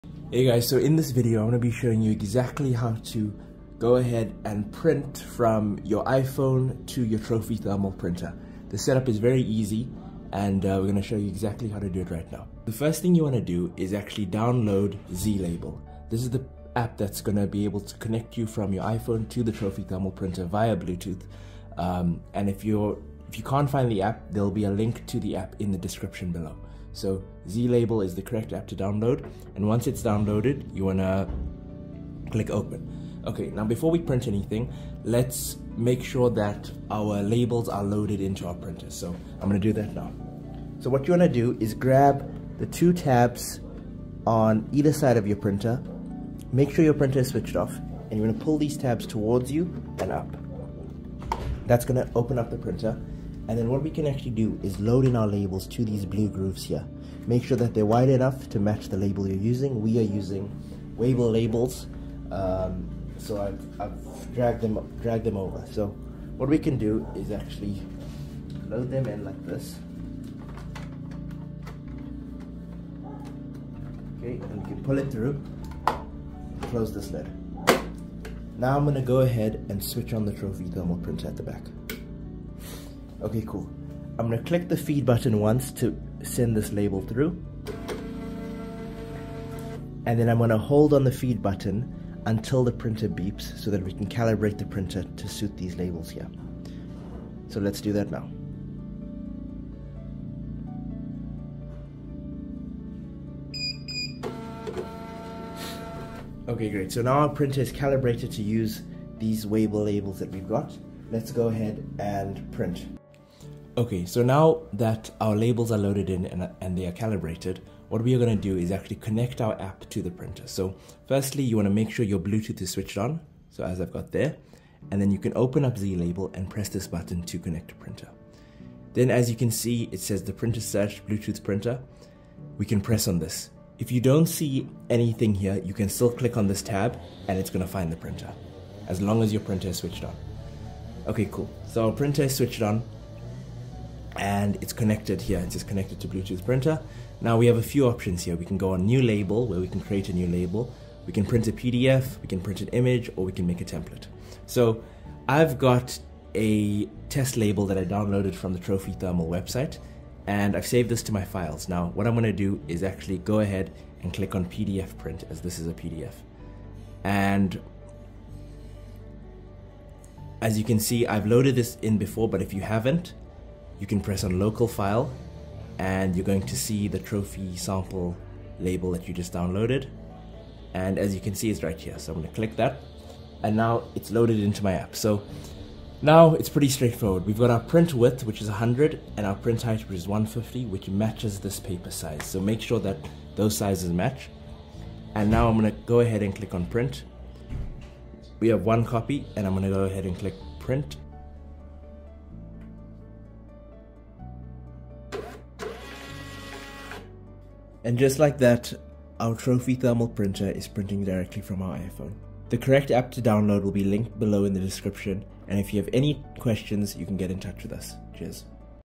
Print from an iOS device with the Trophy Thermal Printer
In this video we show you how to print from your iPhone or iPad directly to the Trophy thermal printer.